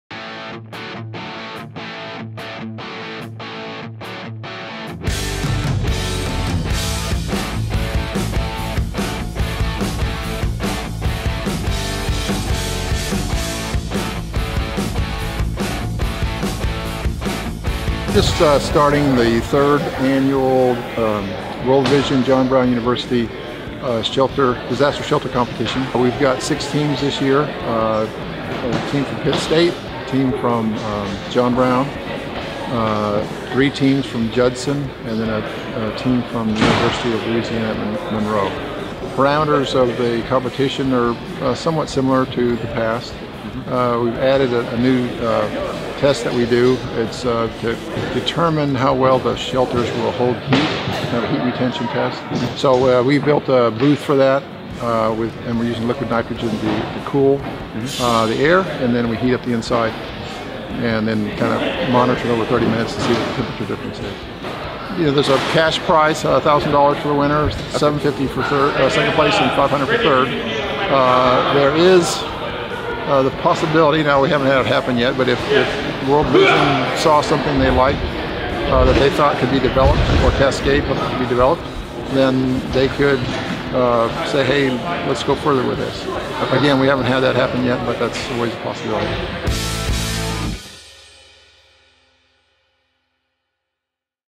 2014 World Vision Disaster Shelter Design Competition
John Brown University hosts the annual 2014 World Vision Disaster Shelter Design Competition